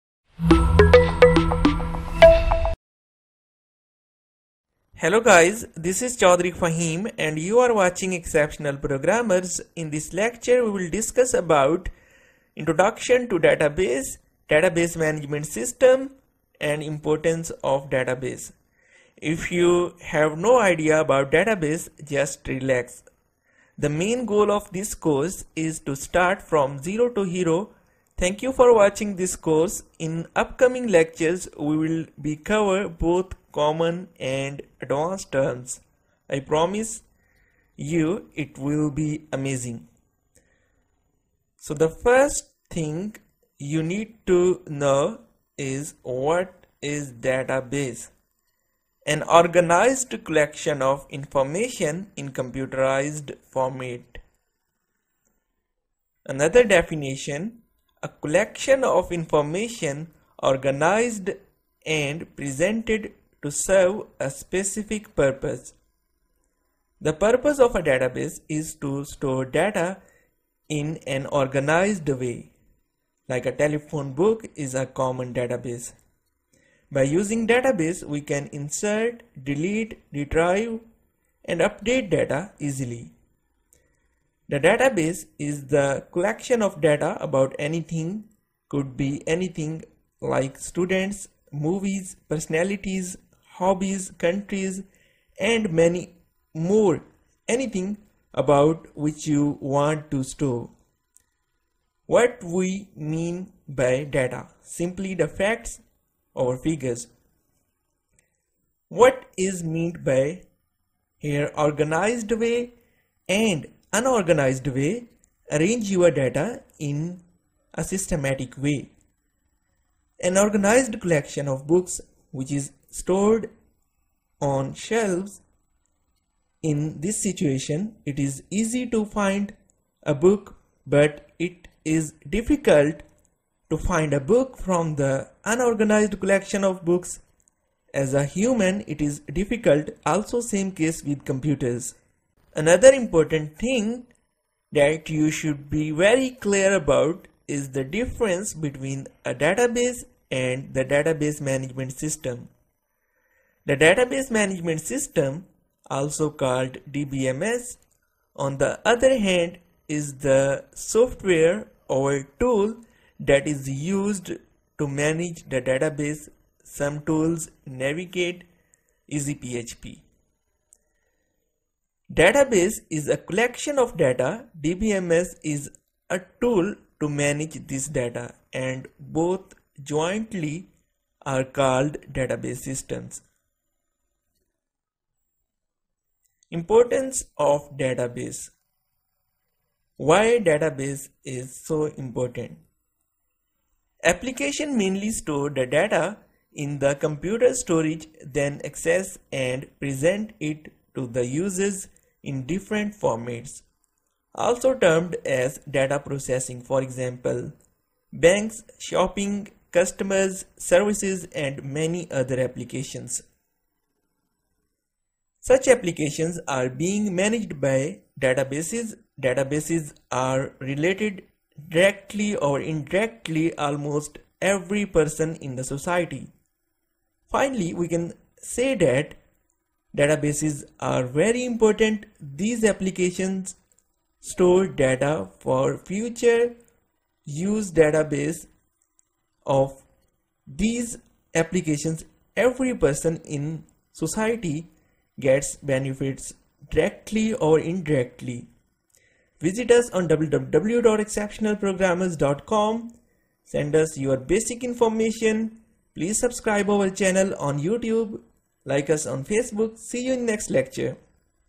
Introduction to database, DBMS vs Database and importance of database
introduction to database, database management system & importance of database.

This course is first (fundamental to advance) course on database management systems and structured query language.